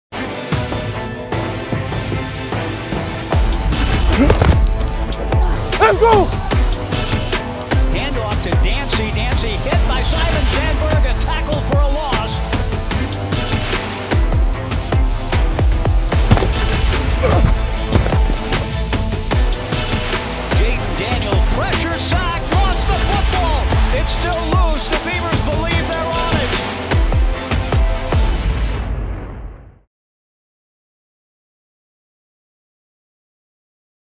2020 Preview: Defensive Line Hype
The Beavers return play makers on the defensive line and will look to reek havoc on opposing quarterbacks this year.